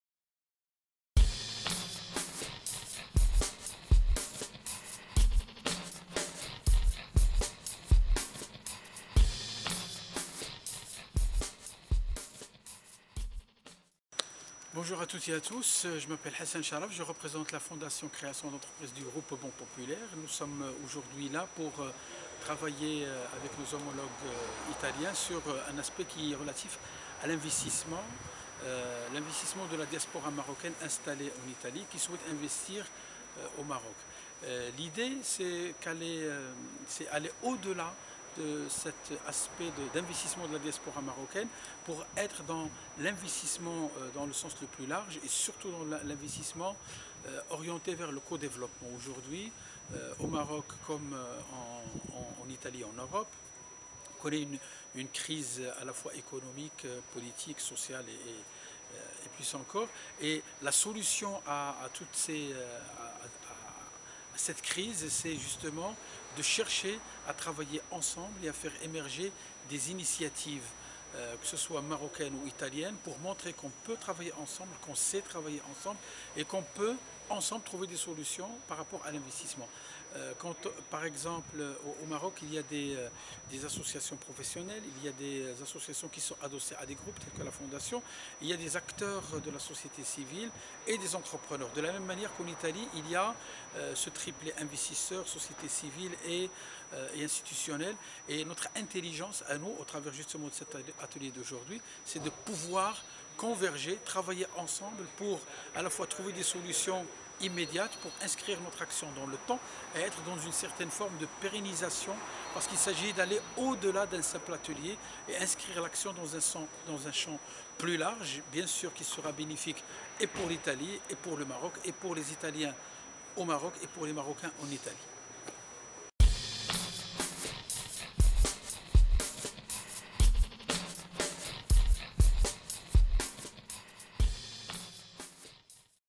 Intervista a Hassan Charraf in occasione di MEDEA
Intervista a Hassan Charraf, Capo dipartimento sviluppo della Fondazione Creazione d'Impresa della Banca Popolare del Marocco, in occasione di MEDEA, Co-Sviluppo tra Milano e Marocco: opportunità e prospettive future, 24/25 giugno 2013.